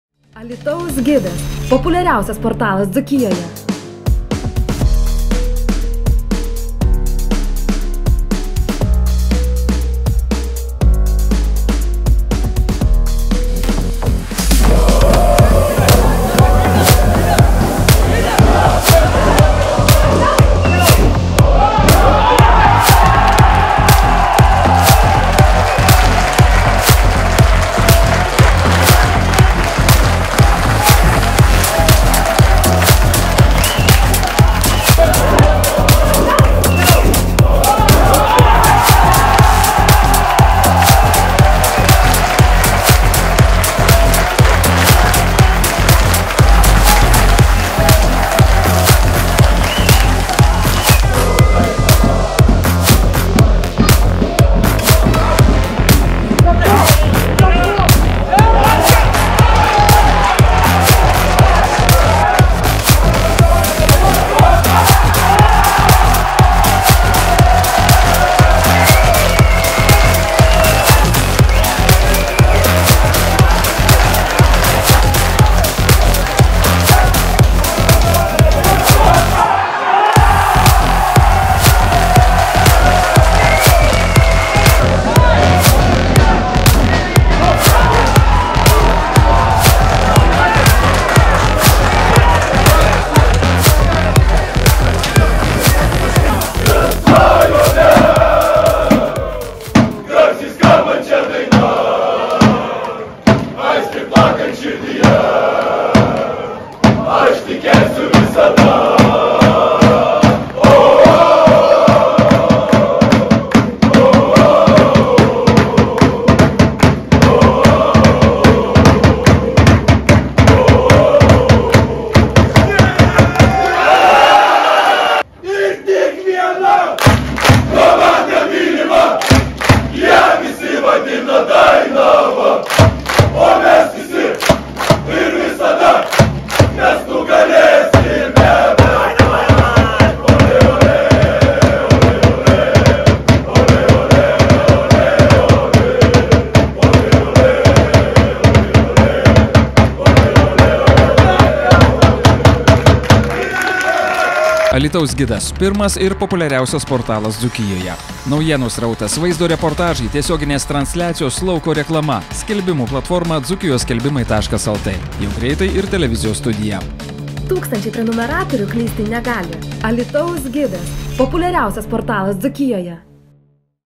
Du įvarčius lygos lyderiams įmušęs Henrique Sarmengue Devens šėlo su „Dzūkų tankais"
Spauskite „AČIŪ“ ir tiesiogiai paremkite kanalą Alytaus gidas. Ačiū :)